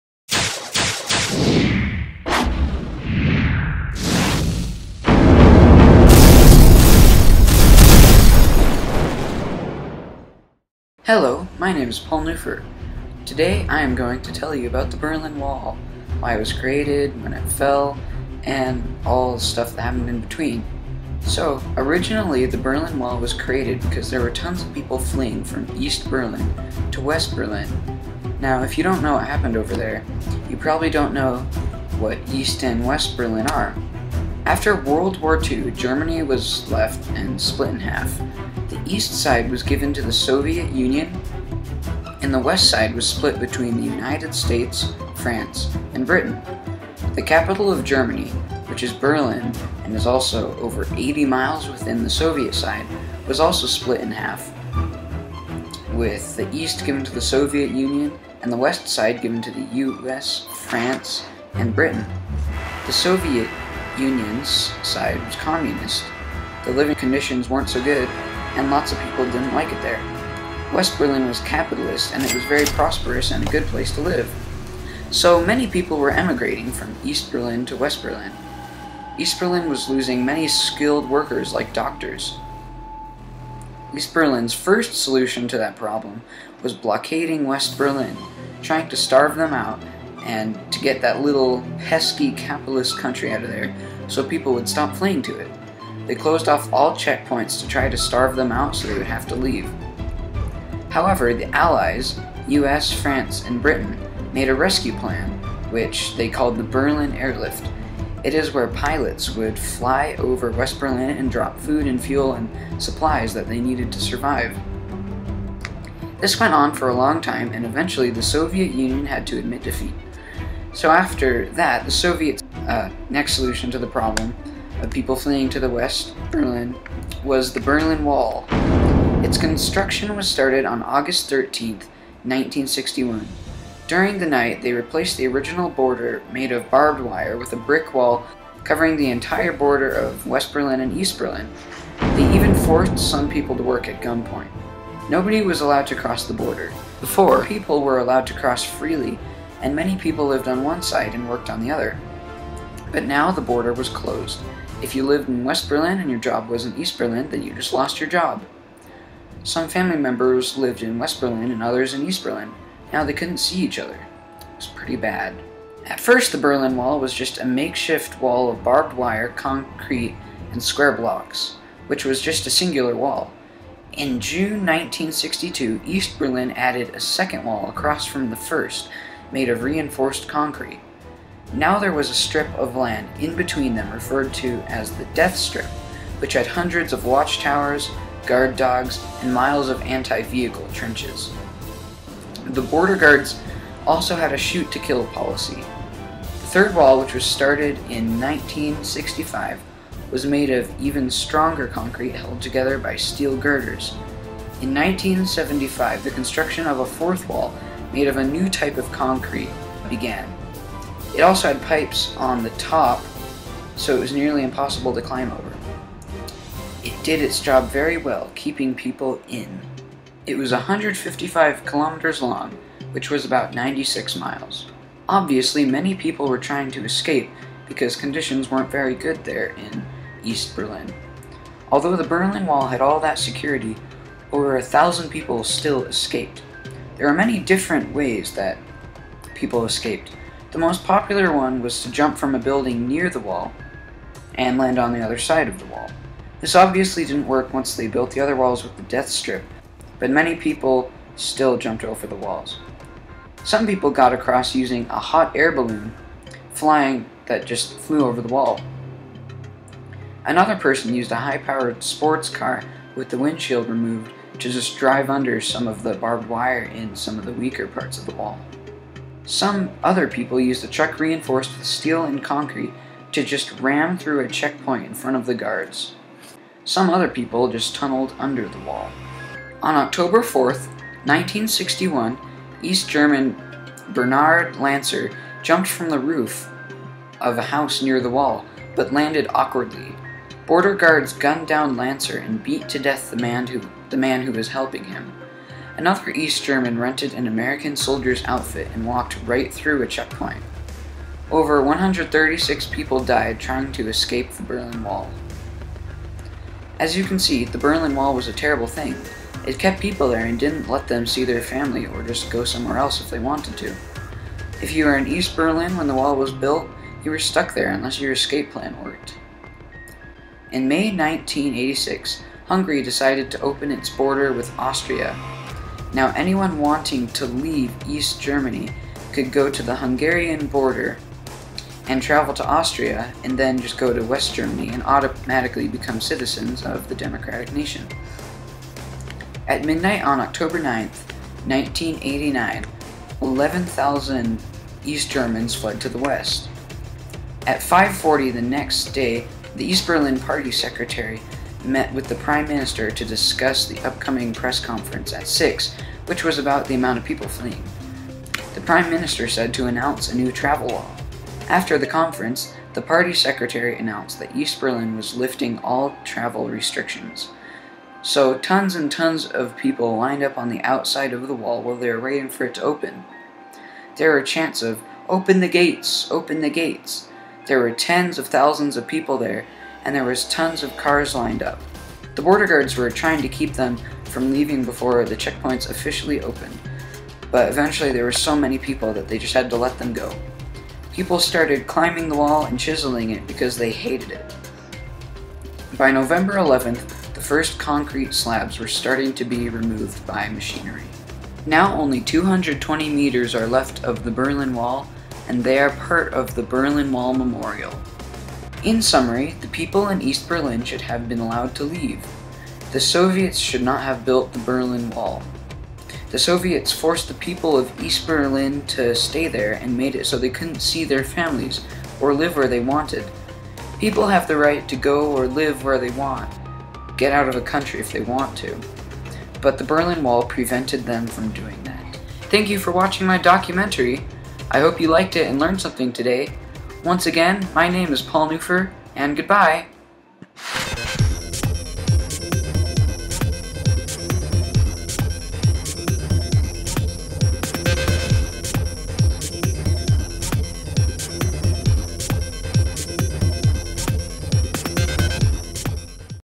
The Rise and Fall of The Berlin Wall Full NHD Documentary
Made for the 2014 NHD History fair topic: Rights and Responsibilities